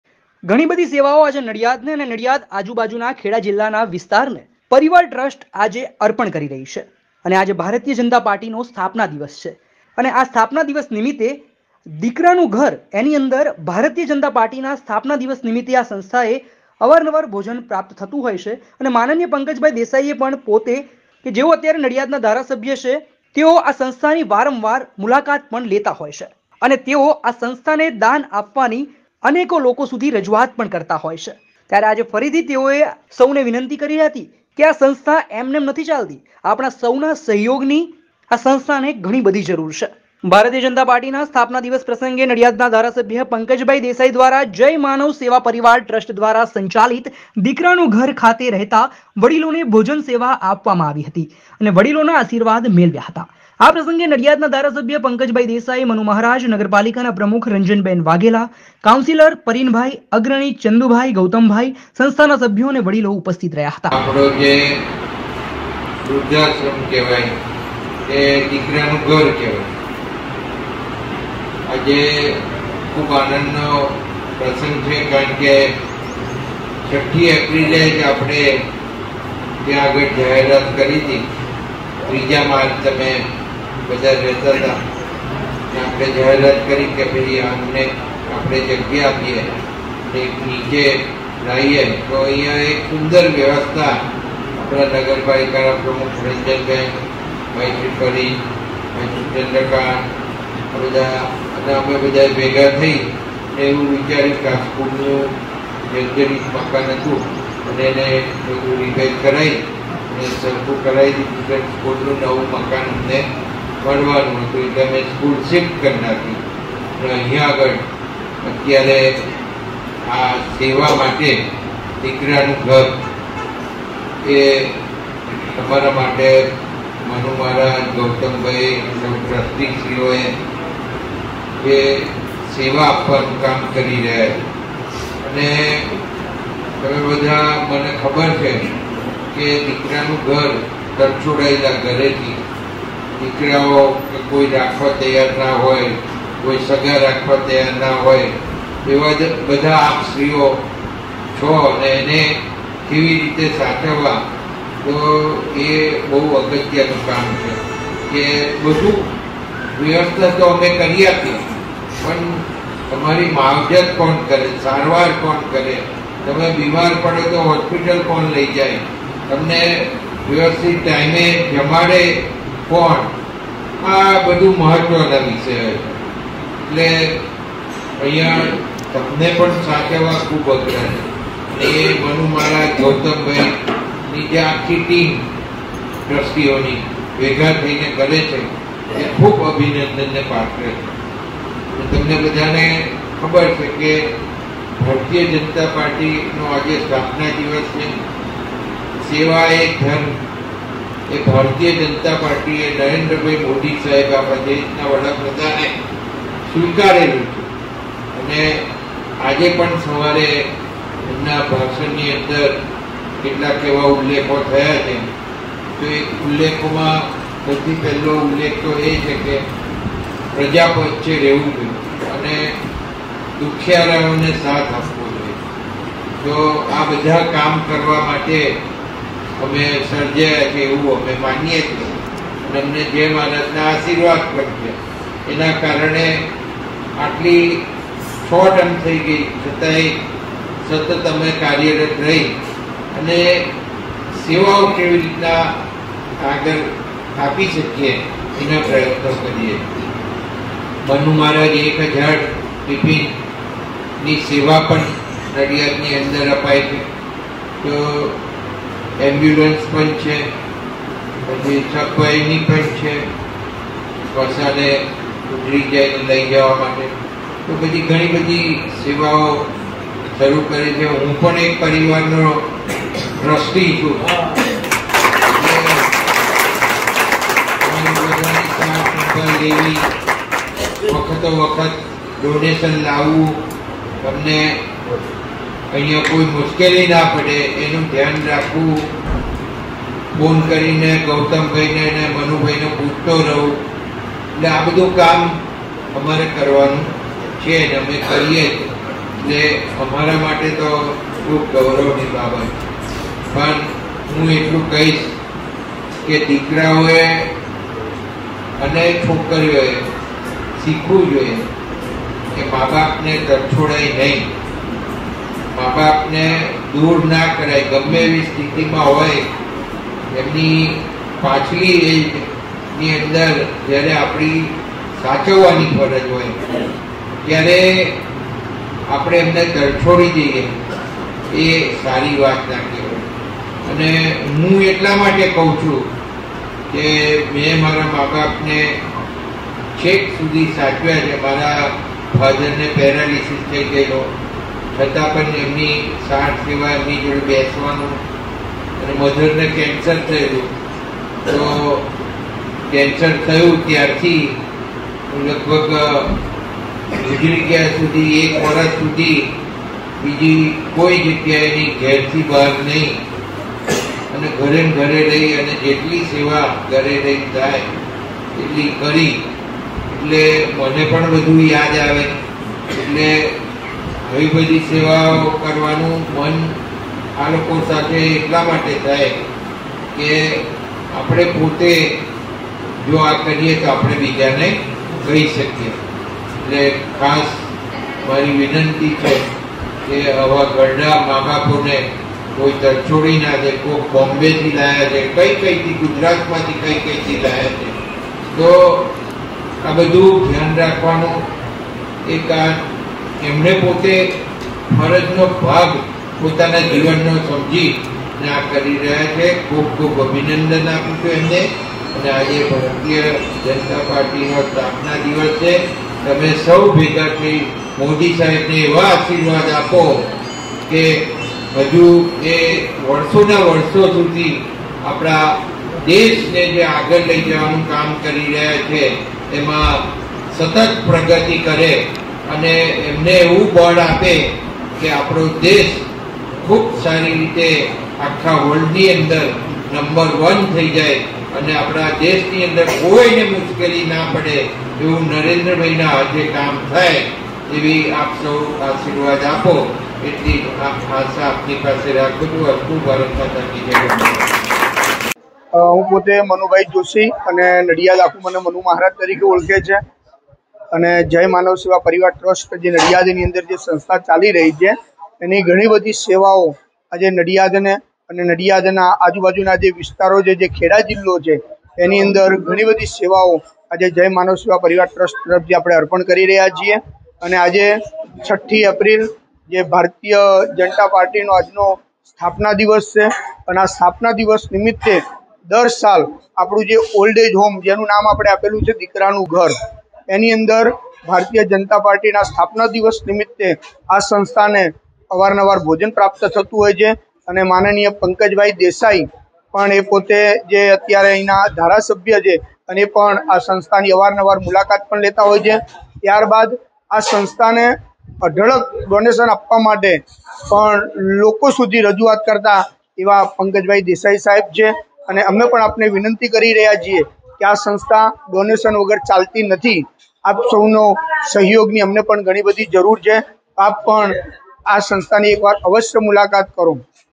NADIAD : BJP સ્થાપના દિવસ નિમિત્તે સંસ્થાને ભોજન પ્રાપ્ત
NADIAD : 7-4-2023 FRI
ઘણી બધી સેવાઓ આજે નડિયાદને અને નડિયાદના આજુબાજુના જે વિસ્તારો જે ખેડા જિલ્લો છે એની અંદર ઘણી બધી સેવાઓ પરિવાર ટ્રસ્ટ આપણે અર્પણ કરી રહ્યા છીએ અને આજે ભારતીય જનતા પાર્ટીનો આજનો સ્થાપના દિવસ છે અને આ સ્થાપના દિવસ નિમિત્તેજ હોમ જેનું નામ આપણે આપેલું છે દીકરા નું ઘર એની અંદર ભારતીય જનતા પાર્ટીના સ્થાપના દિવસ નિમિત્તે આ સંસ્થાને અવારનવાર ભોજન પ્રાપ્ત થતું હોય છે અને માનનીય પંકજભાઈ દેસાઈ પણ એ પોતે જે અત્યારે એના ધારાસભ્ય છે અને પણ આ સંસ્થાની વારંવાર મુલાકાત પણ લેતા હોય છે ત્યારબાદ આ સંસ્થાને આપવા માટે પણ લોકો સુધી રજૂઆત કરતા એવા પંકજભાઈ દેસાઈ સાહેબ છે અને અમને પણ આપણે વિનંતી કરી રહ્યા છીએ કે આ સંસ્થા વગર ચાલતી નથી આપ સૌનો સહયોગની અમને પણ ઘણી બધી જરૂર છે.ભારતીય જનતા પાર્ટીના સ્થાપના દિવસ પ્રસંગે નડિયાદ ધારાસભ્ય શ્રી પંકજભાઈ દેસાઈ દ્વારા જય માનવ સેવા પરિવાર ટ્રસ્ટ દ્વારા સંચાલિત "દીકરાનું ઘર" ખાતે રહેતા વડીલોને ભોજન સેવા આપવામાં આવી હતી અને વડીલોના આશિર્વાદ મેળવ્યા હતા.
આ પ્રસંગે નડિયાદના ધારાસભ્ય શ્રી પંકજભાઈ દેસાઈ , શ્રી મનુભાઈ મહારાજ, નગરપાલિકા પ્રમુખ શ્રી રંજનબેન વાઘેલા, કાઉન્સિલર શ્રી પરિનભાઈ, અગ્રણી શ્રી ચંદુભાઈ, શ્રી ગૌતમભાઈ, સંસ્થાના સભ્યો, અને વડીલો ઉપસ્થિત રહ્યા હતા.